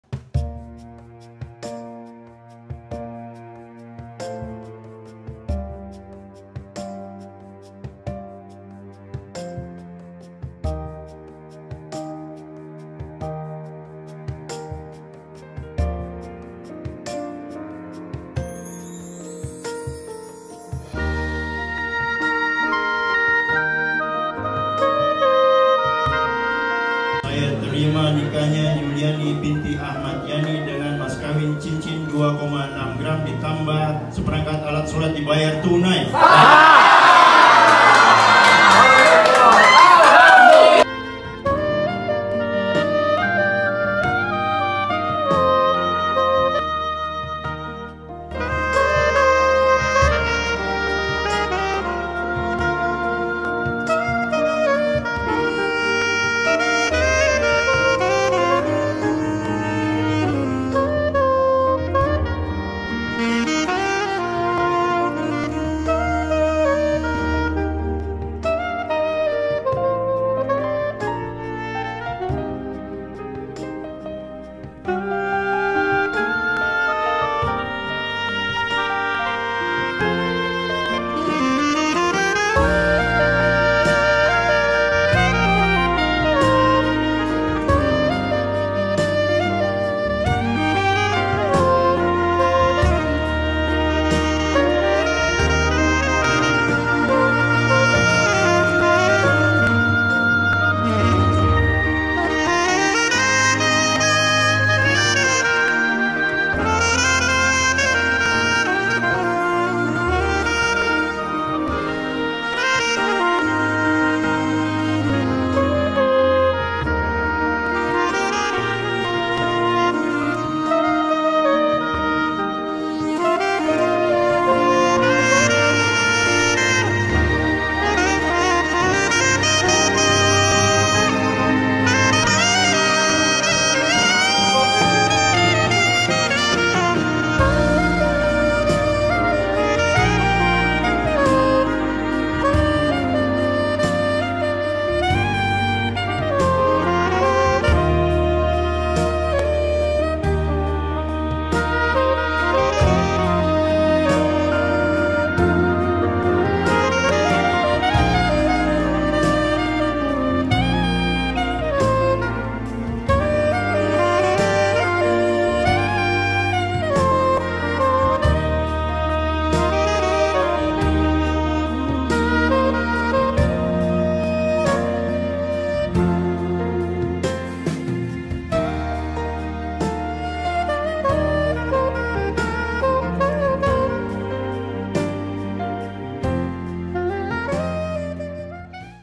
WEDDING AT THE CAFE 🥰🥰 @kedaibcb9278 "HAPPY WEDDING ❤️RACHMAT & YUNA❤️"
"Selamat atas Pernikahan kalian , semoga Pernikahan Kalian Senantiasa di Limpah kan Berkah , Cinta Kebaikan Dan Kebahagiaan" .
AMIN. 🙏🙏🙏

Terimakasi yang Tak Terhingga sudah melangsungkan AKAT NIKAH yang sungguh indah Dan Penuh Cinta di kedai BCB..

Salam Hormat kami ..